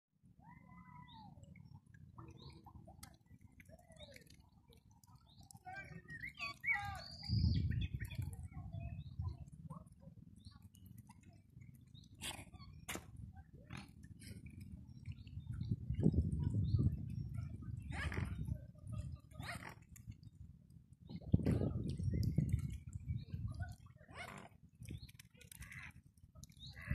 Black Swan in the middle surrounded by white swans
Black Swans are very rare in England.

Location : Princess Park BN22, Eastbourne East Sussex UK ⛲🏞️
Time : at 8 o'clock in the evening on the 23rd of May 2022